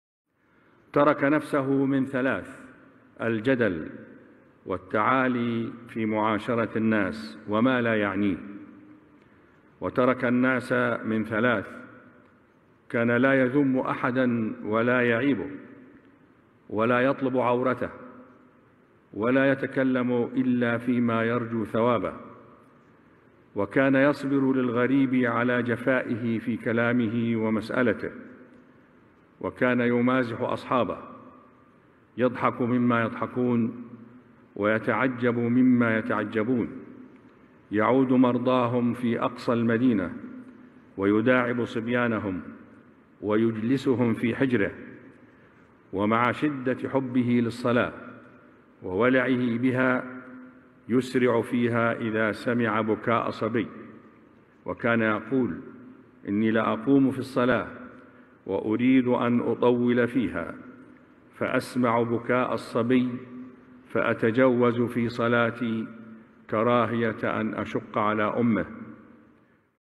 ترك - صلى الله عليه وسلم- نفسه من ثلاث: الجدل، والتعالي في معاشرته الناس، وما لا يعنيه..
وترك الناس من ثلاث: كان لا يذم أحدا ولا يعيبه، ولا يطلب عورته، ولا يتكلم إلا فيما يرجو ثوابه..

من كلمة فضيلة الإمام_الأكبر أ.د/ أحمد الطيب، بمناسبة: «الاحتفال بذكرى المولد_النبوي_الشريف 1443هـ -2021م»